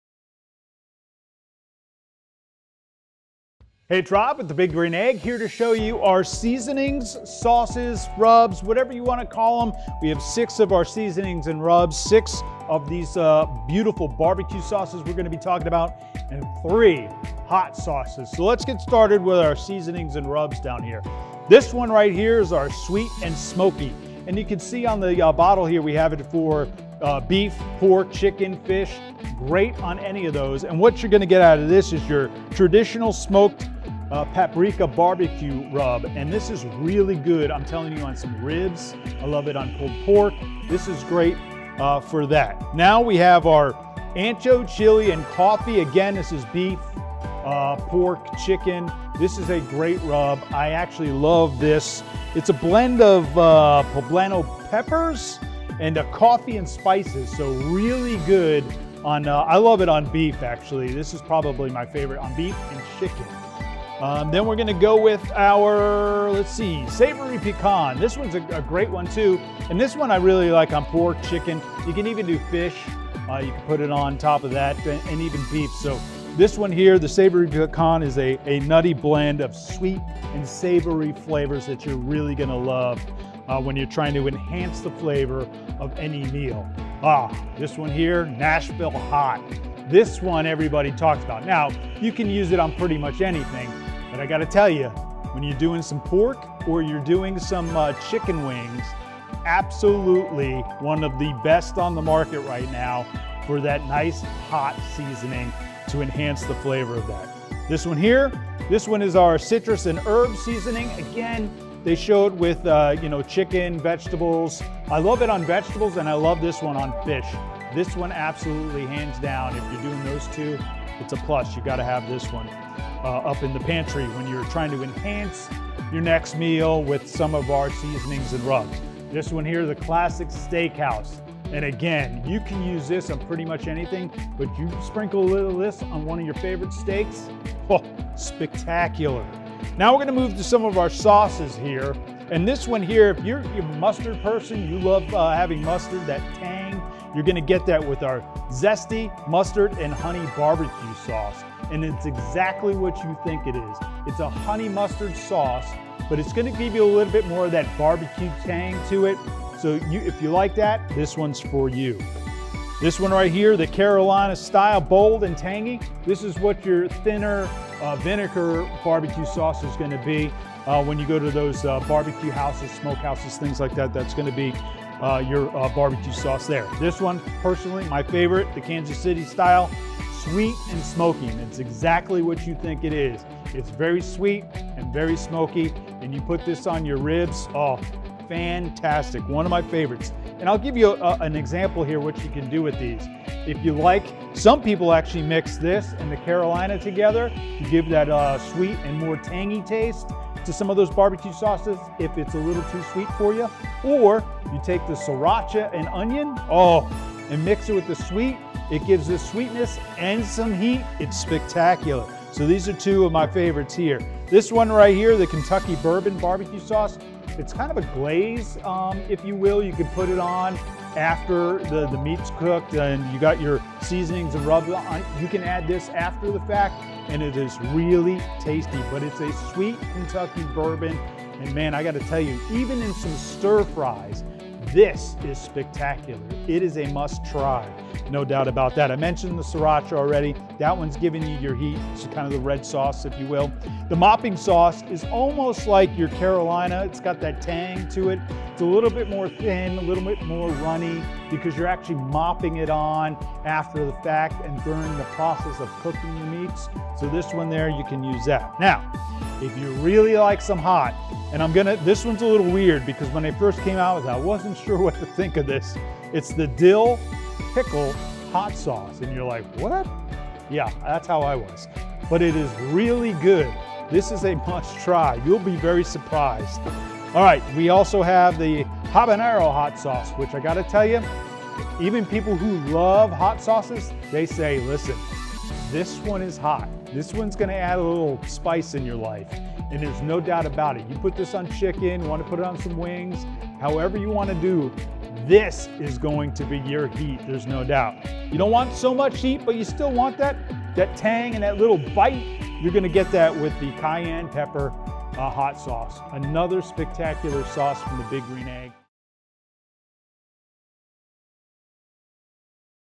Big Bold Seasonings & Sauces from Big Green Egg
Think you’re already making the best steak ever?
The best chicken? salmon? ribs? or brisket?

Think again.

We have 12 Big Green Egg blended seasonings and sauces specifically crafted to enhance any meal you can imagine.

Watch our video below and find the perfect pairing for your next meal on the Big Green Egg!